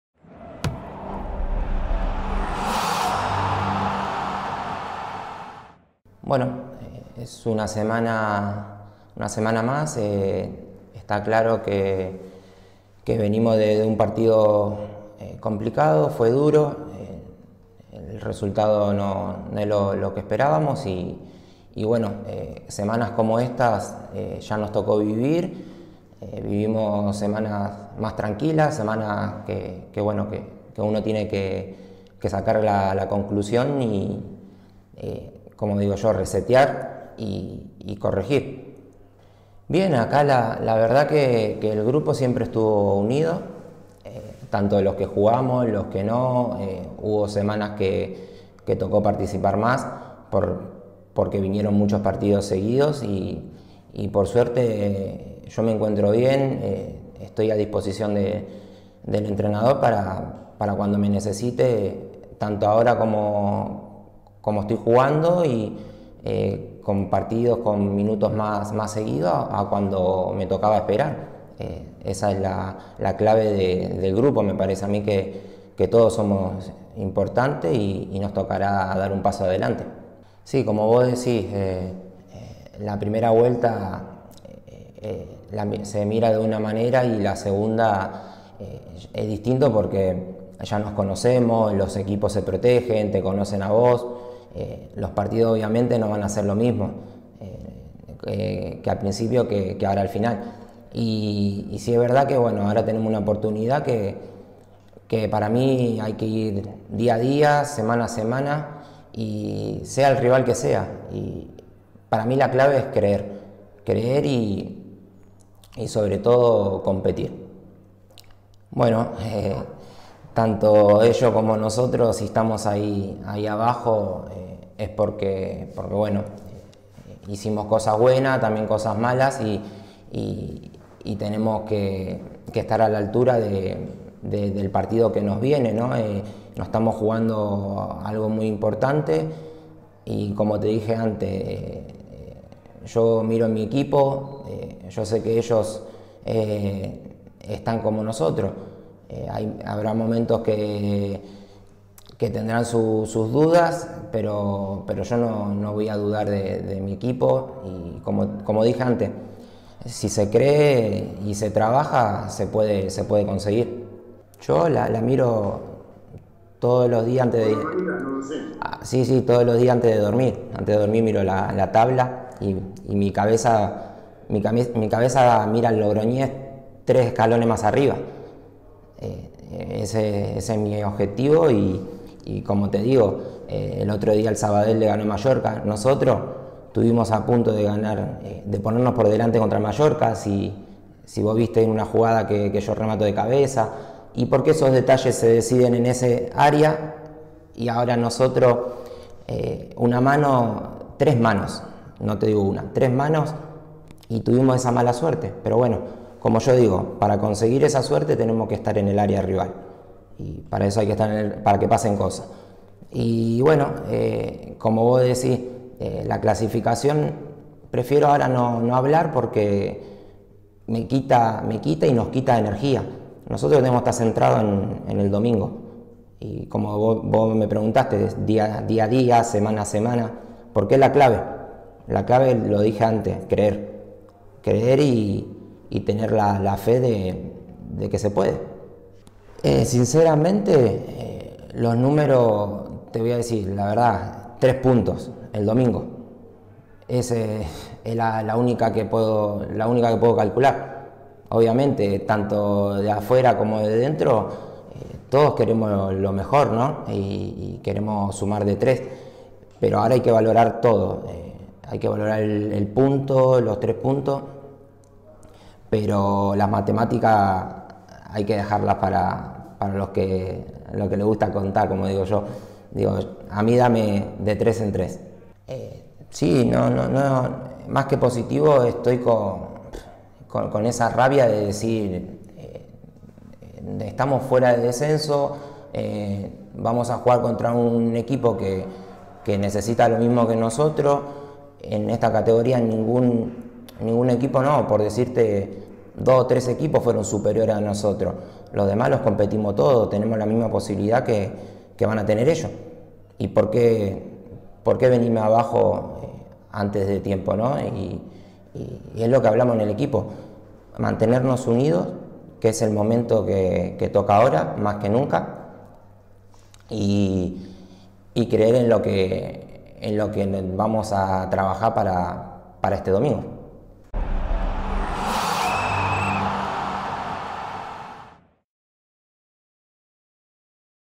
Damián Petcoff: "Esta es de esas semanas en las que hay que sacar conclusiones, resetear y corregir"
El centrocampista argentina ha atendido a los medios de comunicación en la rueda de prensa previa a la jornada 37, en la que la Unión Deportiva Logroñés se enfrentará al CD Castellón en Castalia, en un partido vital para la permanencia en LaLigaSmartBank.